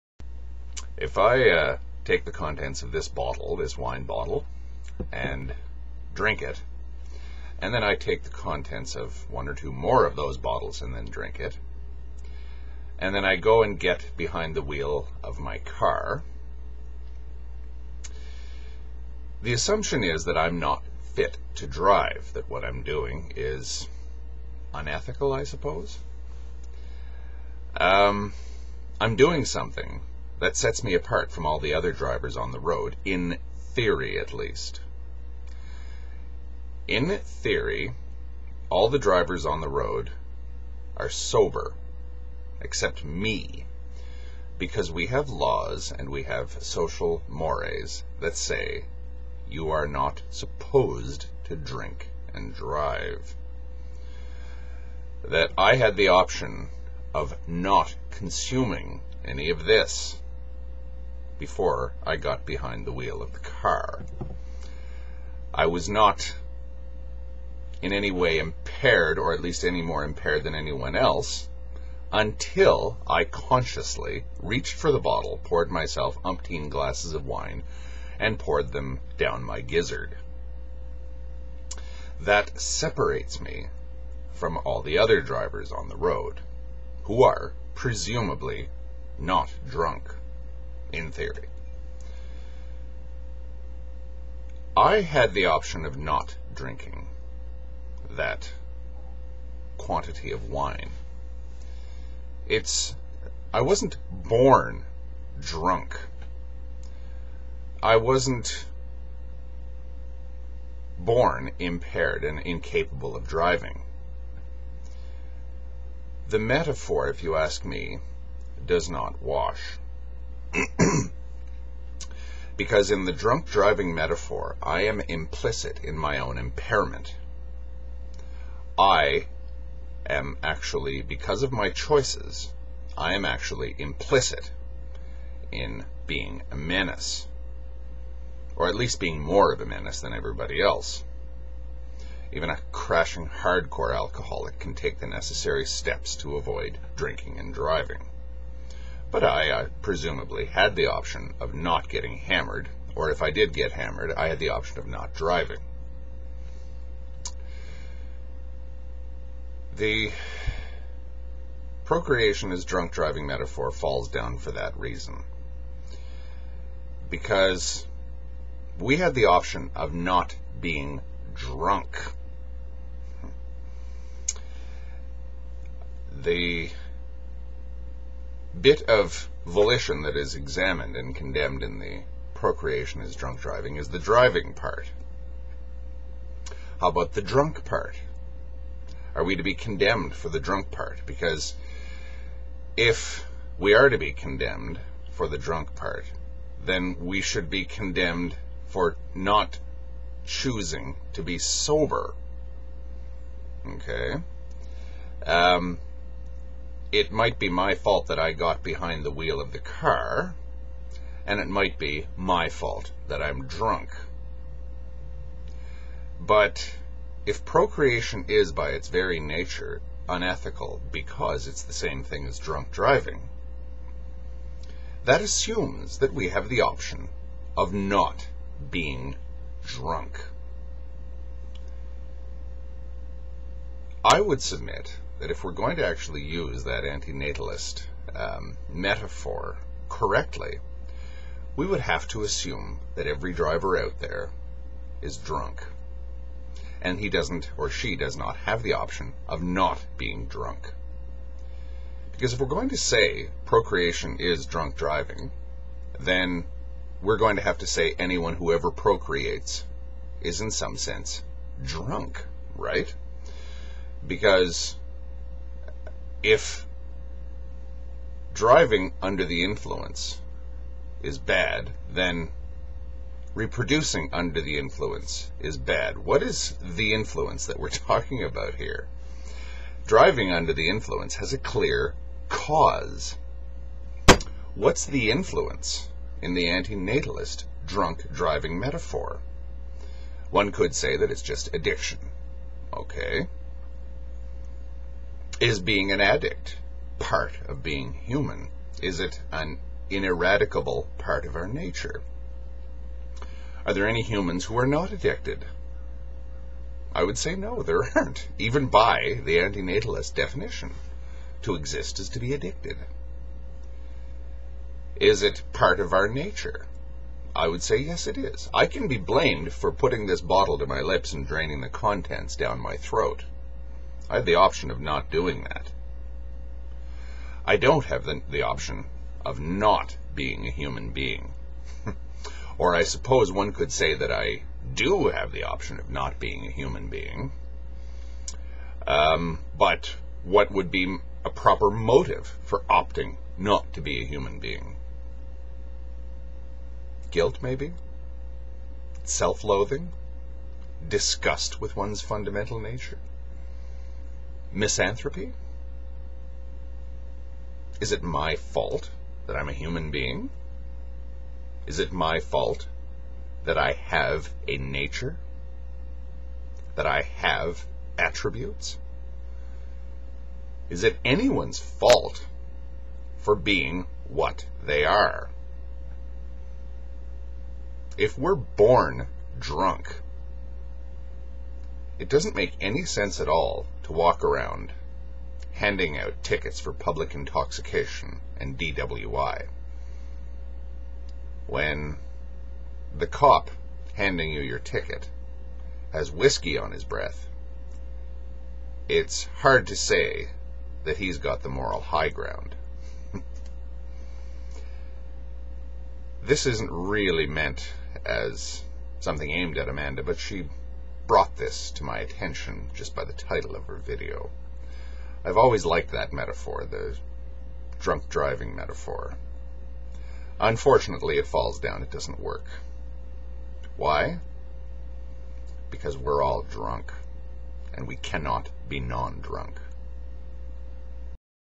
Affirmative: Human = Drunkard (Procreation as DWI)
In the "procreation is drunk driving" metaphor, what is the source of our drunkenness? I would say that it is not something we wish for or seek. Our drunkenness is simply built into our very nature. Now, if drunkenness is simply part of our nature, why should we alter it?

Guilt, perhaps?

We are what we are.